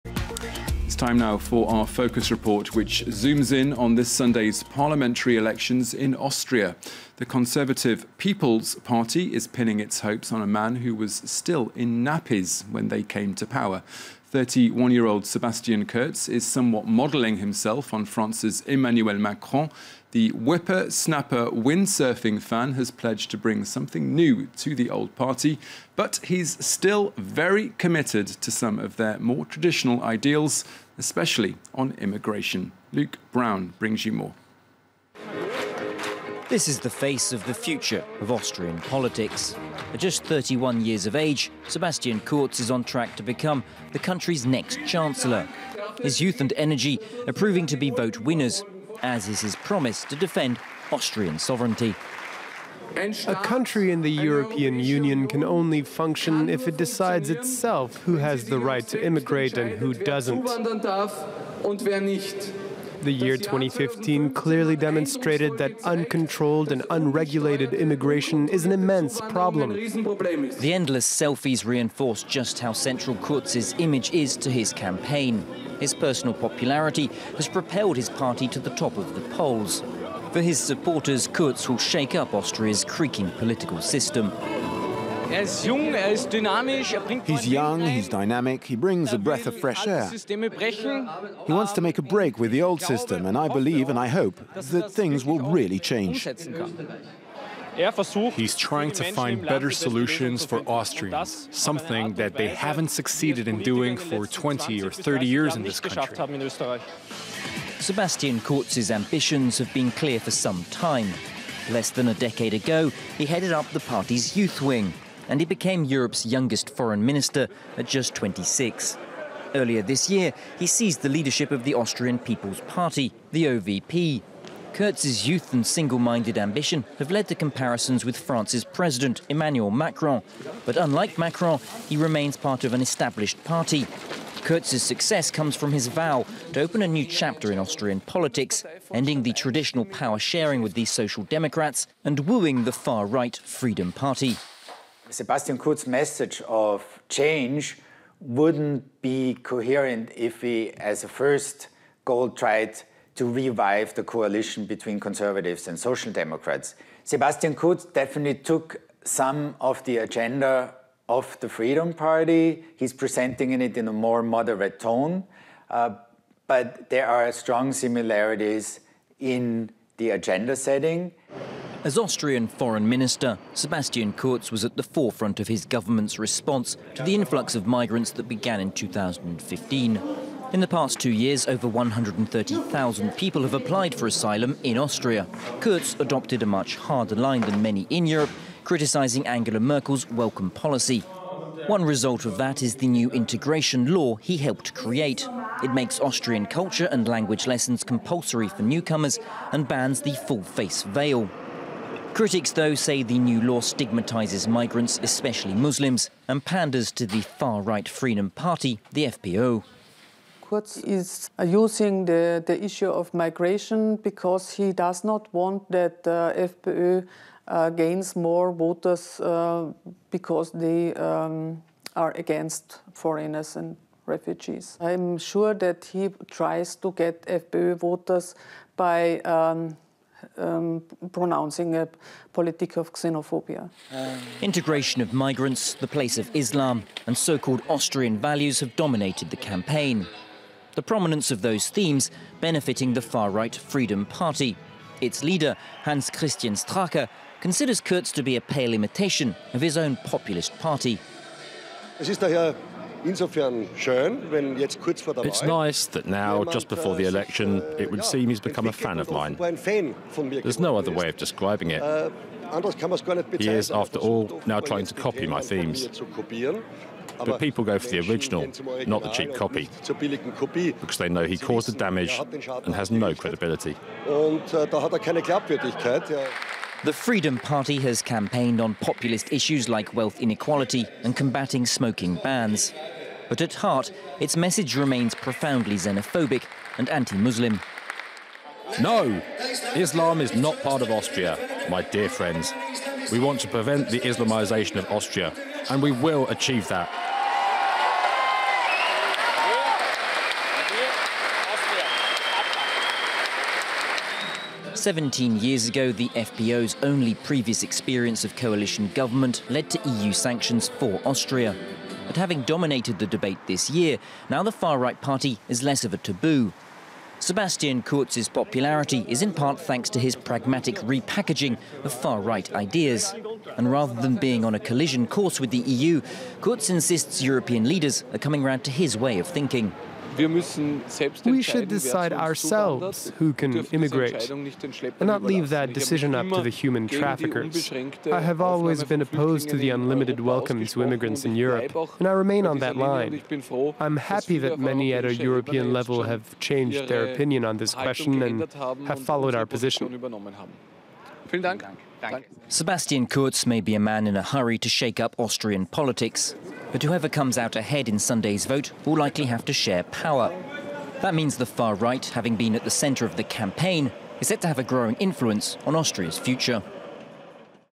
Sebastian Kurz on track to become Austria's next chancellor
He's been dubbed the "Austrian Emmanuel Macron". Sebastian Kurz is only 31 years old, but he's on track to become Austria's new leader when the country heads to the polls this Sunday. The young politician heads up the centre-right People's Party. But unlike France's Macron, Kurz has taken a hard line on immigration, wooing more and more voters by veering sharply to the right. FRANCE 24 reports from Austria.
A programme prepared by Patrick Lovett, François Wibaux and Florence Viala.